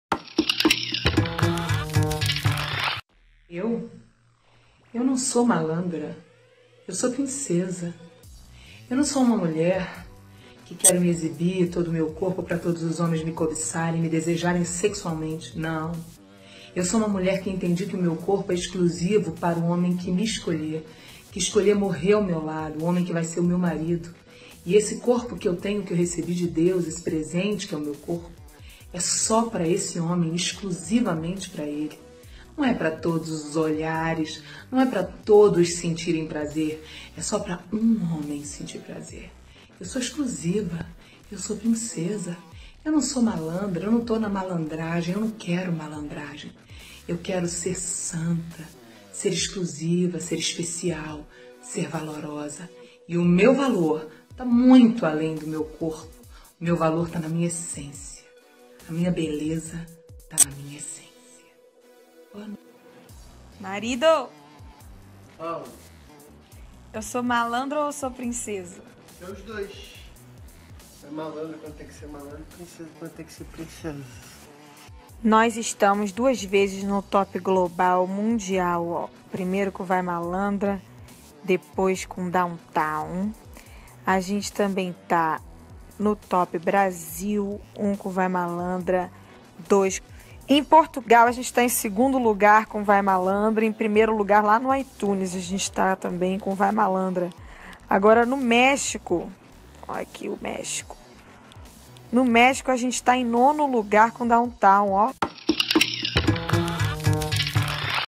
Anitta rebate Sarah Sheeva com pergunta ao marido, Thiago Magalhães
Todo conteúdo desde videograma é de teor jornalistico
e sobre fatos reais e enviado para nossa produção